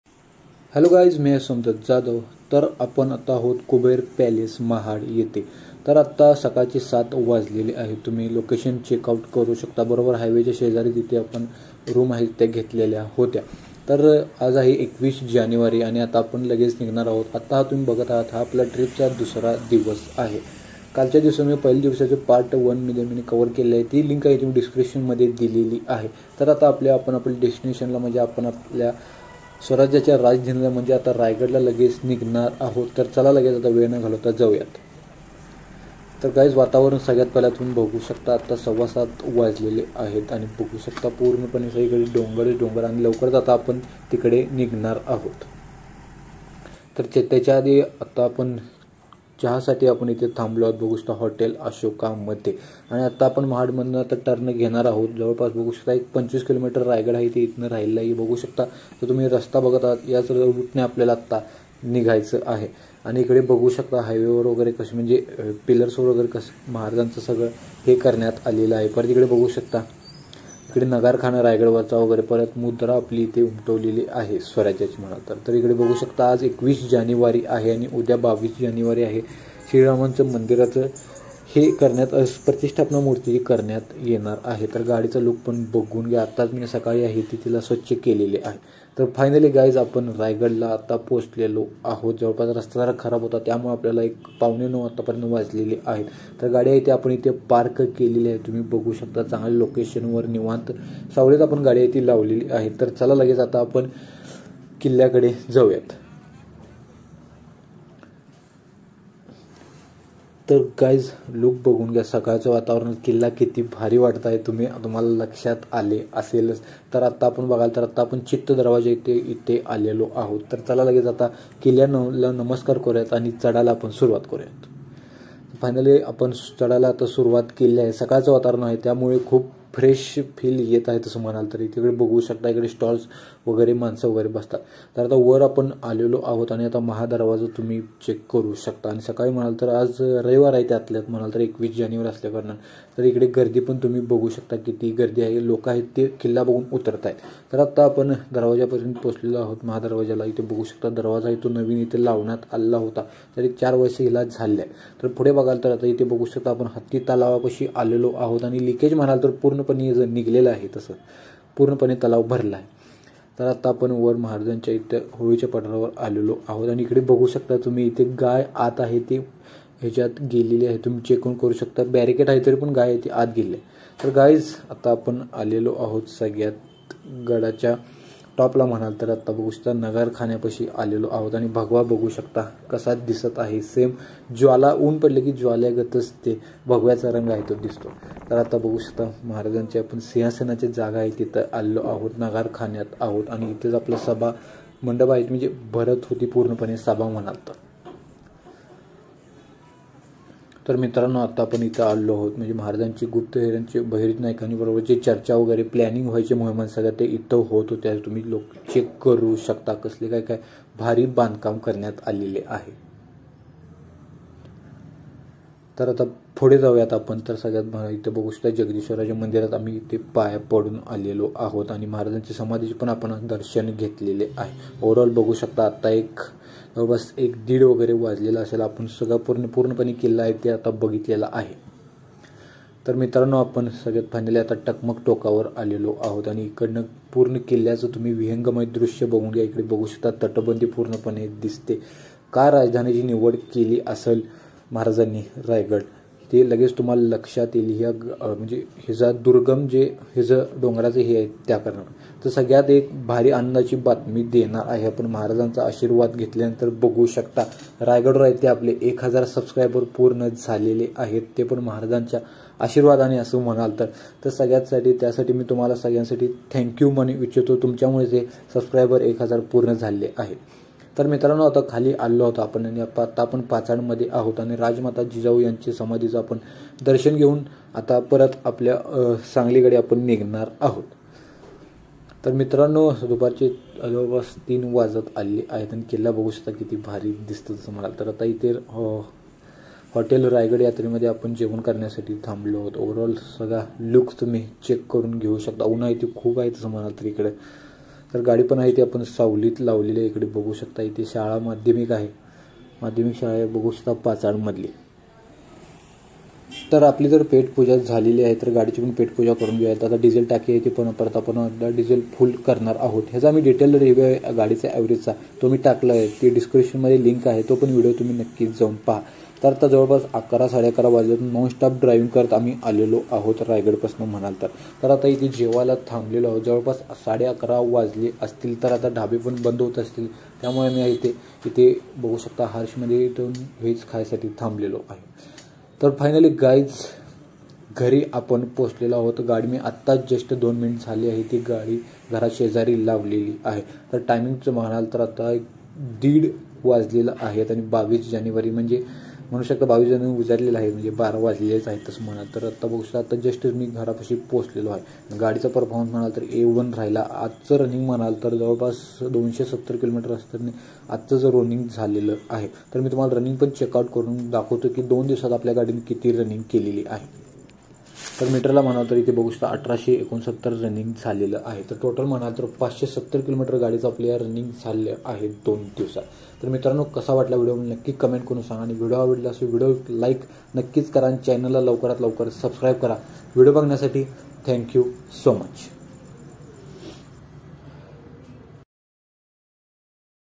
💥 4×4 Jeep घेऊन धोकादायक सहल 💥 || Part 2 || Raigad, Mahabaleshwar
Hello guys,
                    This is our first trip with our new modified Willyz jeep. In this video you see the beauty of Sahyadri. This is part 2.